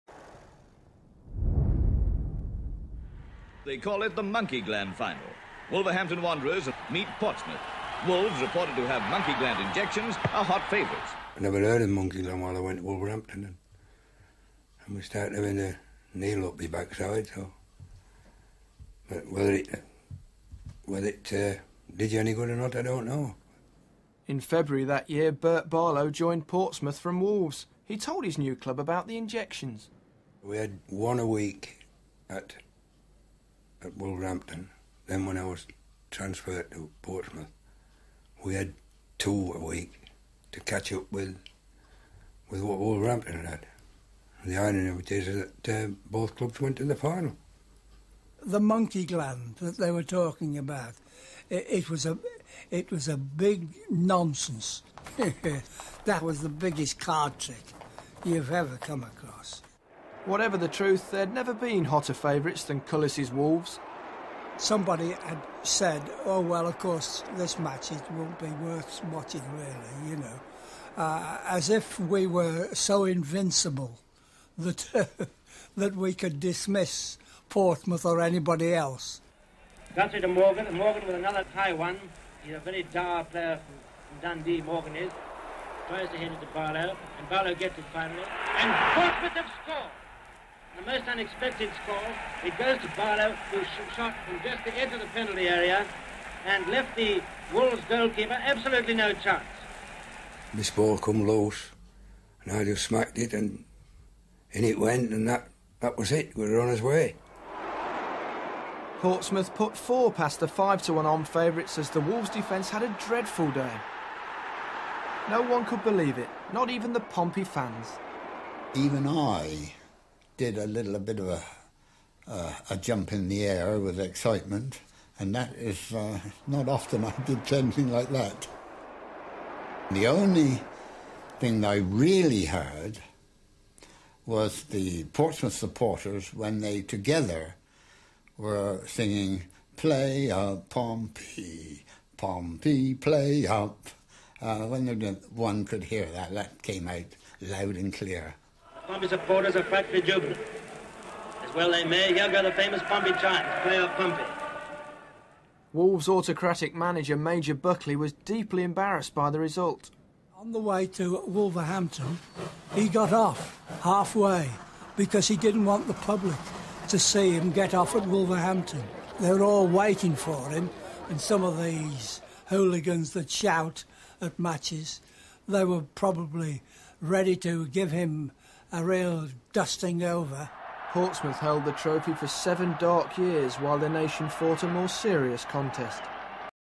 PORTSMOUTH FC V WOLVERHAMPTON WANDERERS FC - FA CUP FINAL 1939 - WEMBLEY - LONDON
Portsmouth FC beat Wolverhampton Wanderers FC 4-1 in the 1939 FA Cup Final played at Wembley Stadium, London on the 29th April. This was the last FA Cup Final played before the Second World War. This video includes recollections of the final from Bert Barlow of Portsmouth and Stan Cullis of Wolverhampton Wanderers. Portsmouth Scorers: (Cliff Parker 2, Bert Barlow, Jon Anderson) Wolverhampton Wanderers Scorer: (Dicky Dorsett).